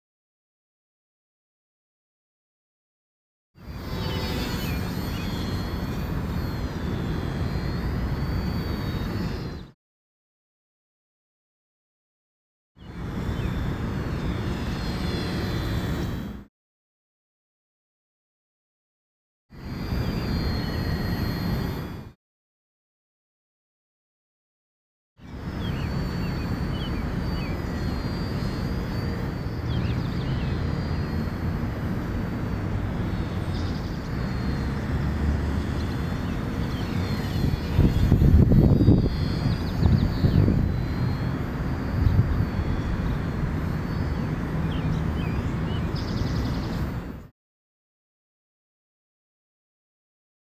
Half scale Polaris photos and brief flight
This is a Polaris shrunk down for the guts from a Parkzone ultra micro and dollar tree foam. Look here for more information on the Polaris